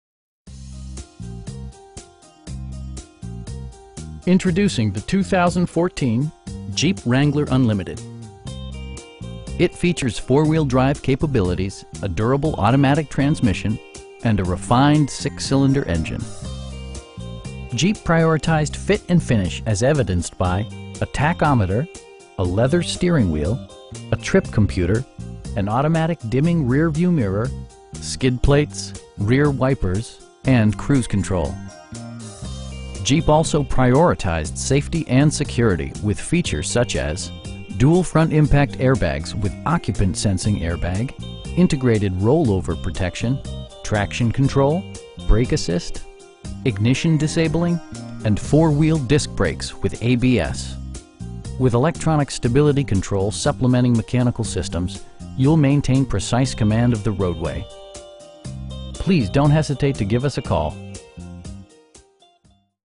2014 Jeep Wrangler Unlimited Sport in Odessa, TX 79761
Introducing the 2014 Jeep Wrangler Unlimited. It features four-wheel drive capabilities, a durable automatic transmission, and a refined 6 cylinder engine. Jeep prioritized fit and finish as evidenced by: a tachometer, a leather steering wheel, a trip computer, an automatic dimming rear-view mirror, skid plates, rear wipers, and cruise control. Jeep also prioritized safety and security with features such as: dual front impact airbags with occupant sensing airbag, integrated roll-over protection, traction control, brake assist, ignition disabling, and 4 wheel disc brakes with ABS. With electronic stability control supplementing mechanical systems, you'll maintain precise command of the roadway. We pride ourselves in the quality that we offer on all of our vehicles. Please don't hesitate to give us a call.